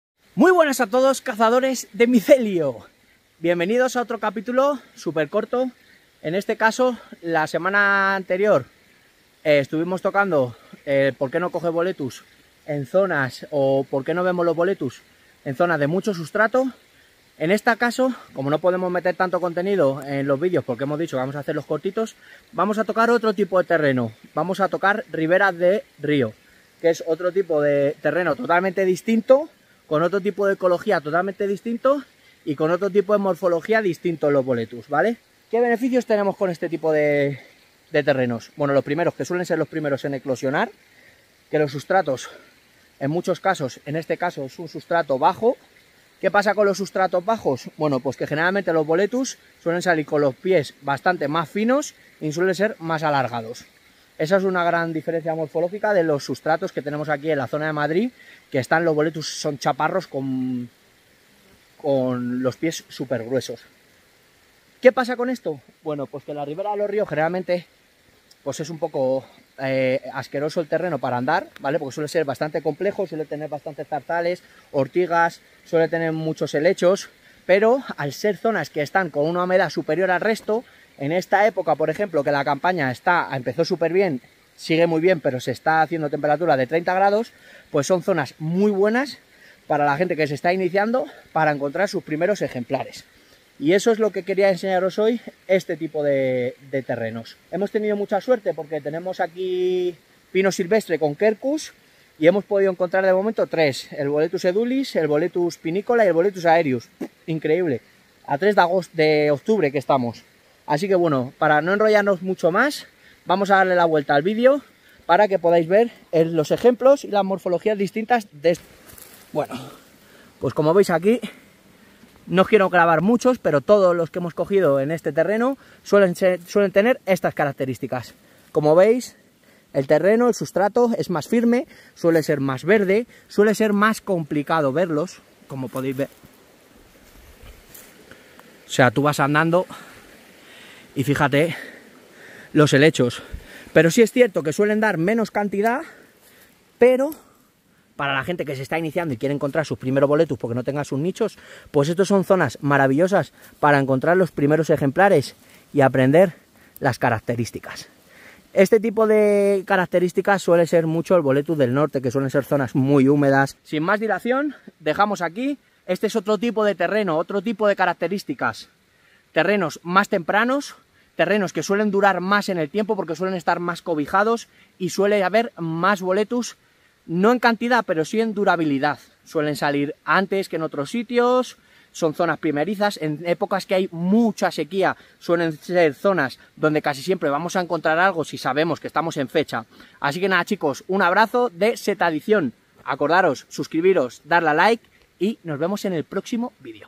Los secretos del Boletus en las riveras de los ríos. Comportamiento en sustrato bajo. Capitulo#2
En esta entrega aprendemos que las riveras de ríos son una zona con un clima especial y unas cualidades únicas. Descubre algo nuevo del mundo de las setas en este capítulo.